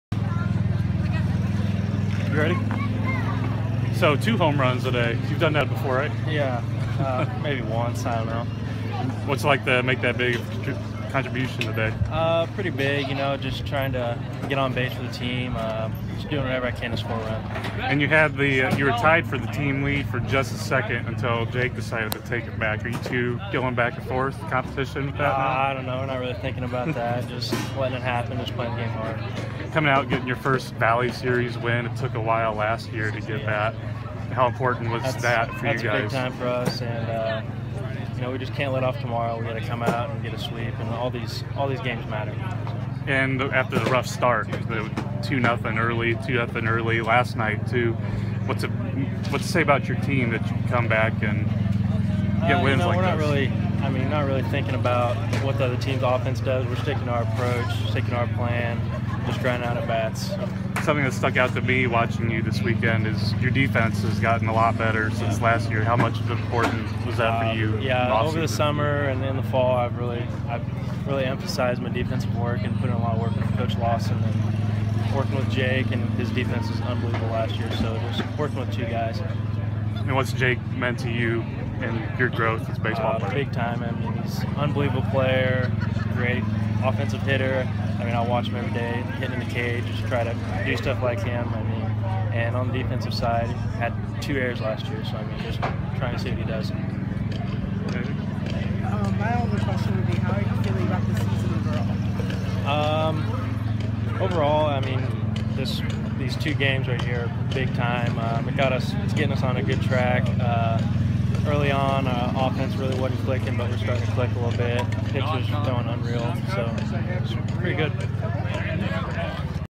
Jeremy Eierman - Baseball Player Postgame Press Conference (April 1)
Shortstop Jeremy Eierman chats about his two home run, four RBI game and helping the Bears to pick up the series win over Bradley at Hammons Field.